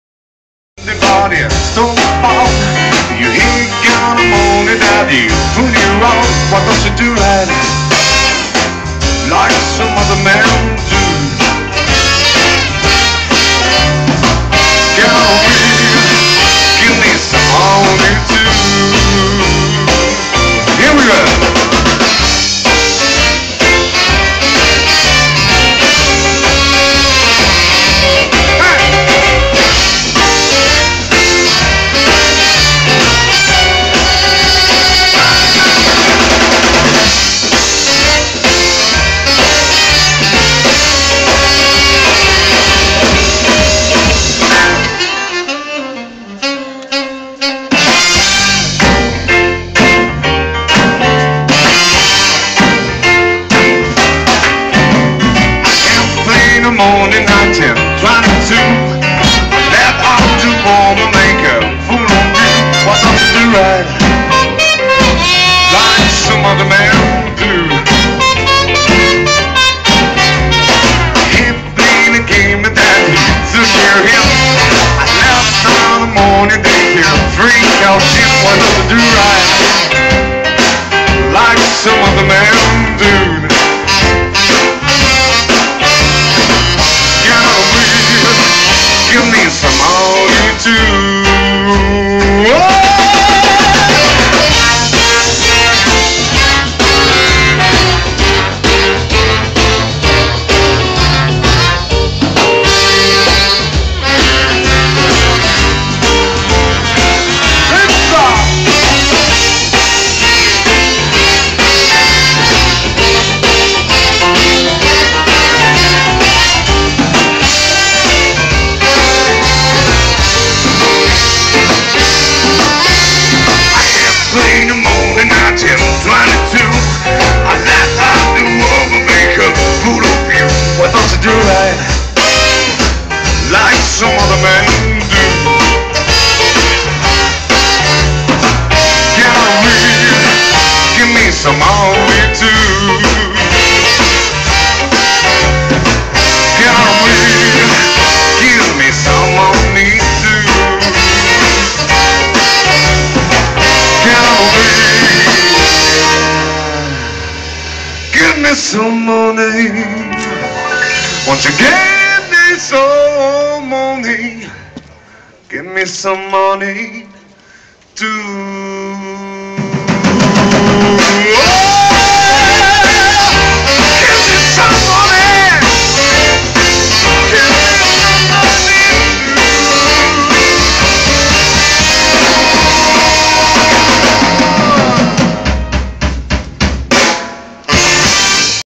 Gli Spaghetti Style swing orchestra a Villa Fondi!
Le atmosfere del jazz e dello swing magistralmente interpretate da questa band campana che ha allietato la rassegna jazz di Piano di Sorrento!